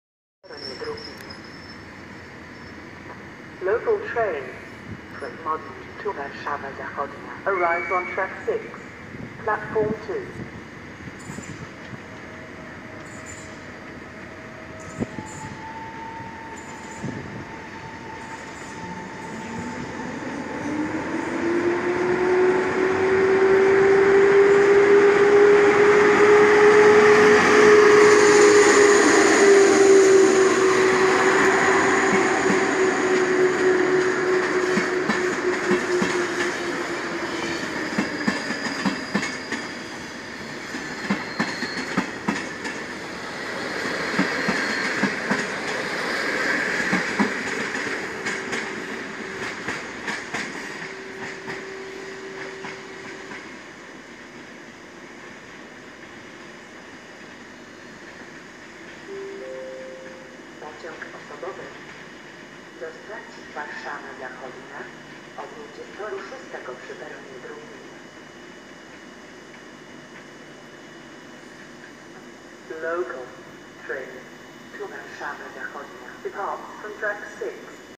EP08-001 + IC "Podlasiak"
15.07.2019. Świnka odjeżdża ze stacji Warszawa Wschodnia na czele pociągu IC"Podlasiak" z Warszawy Zachodniej do Suwałk.